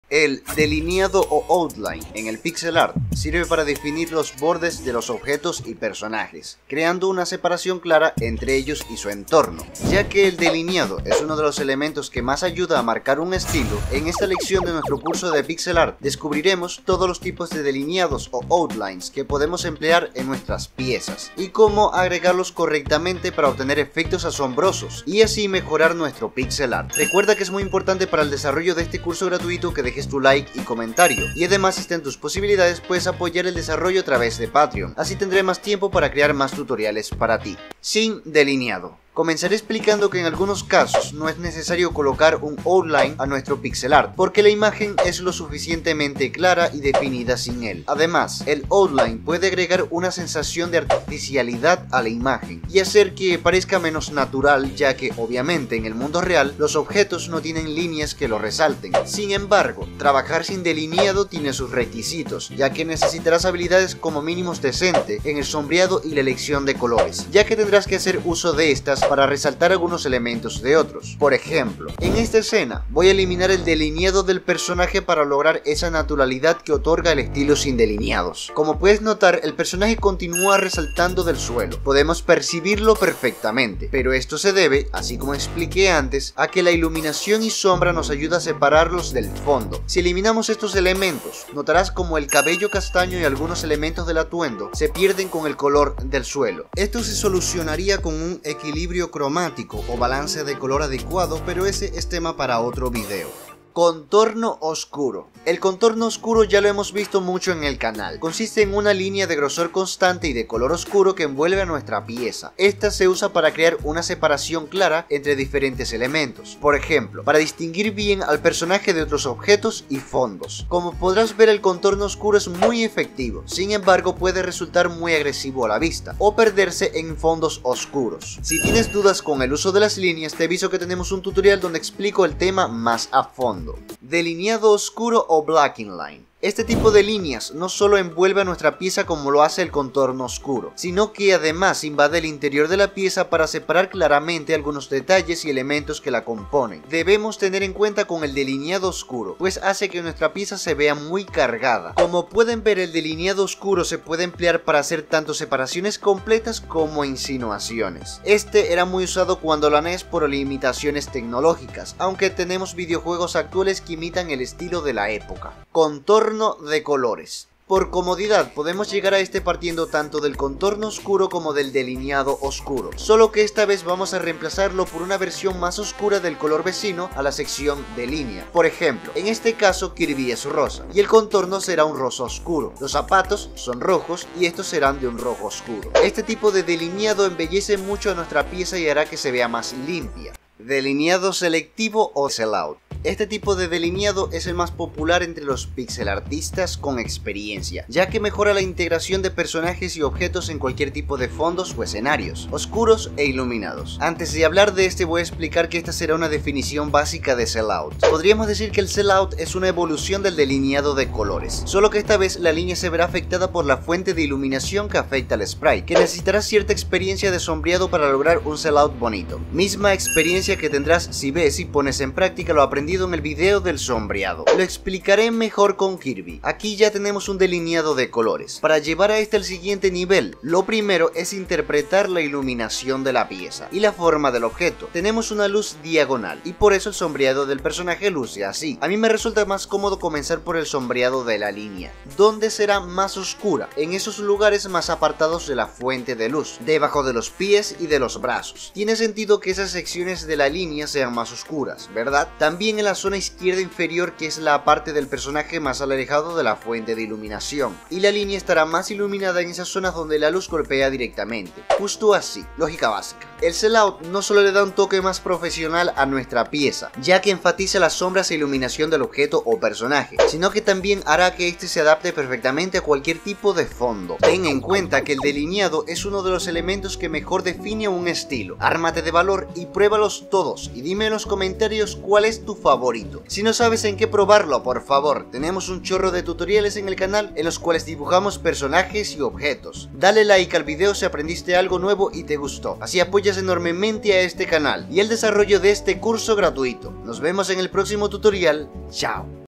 🔴 CÓMO HACER PIXEL ART: Delineados Artísticos.
¡Acompáñanos en este tutorial de pixel art donde exploraremos los diferentes tipos de delineados, incluyendo sin delineado, contorno oscuro, delineado oscuro, delineado de colores y Sel-Out, y aprenderemos cómo aplicarlos correctamente para que nuestras obras resplandezcan de belleza y adquieran diversos estilos. Descubre los secretos para dar vida a tus creaciones en pixel art y llevarlas al siguiente nivel con técnicas de delineado efectivas. No te pierdas esta oportunidad de perfeccionar tus habilidades y destacar en el fascinante mundo del pixel art.